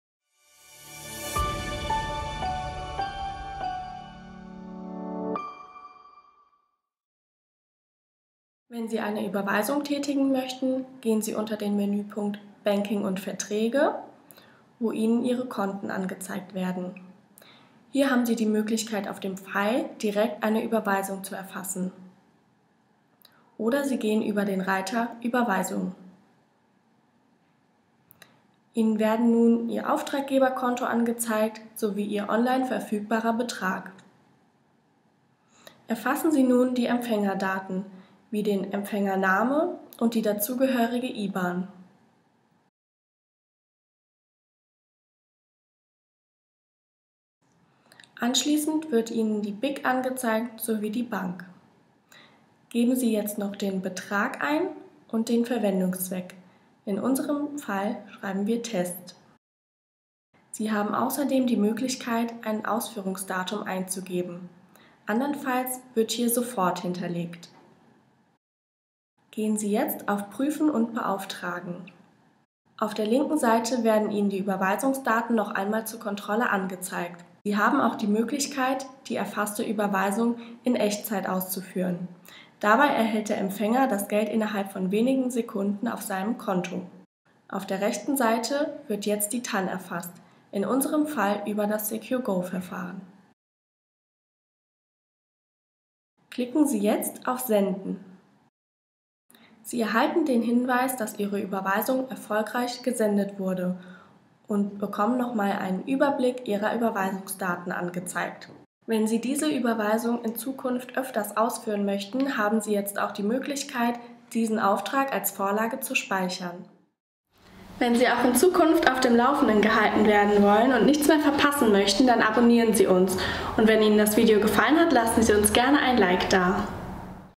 VR OnlineBanking: Überweisung
In diesem Video zeigen wir Ihnen, wie Sie eine Überweisung im neuen VR OnlineBanking erfassen und ausführen können.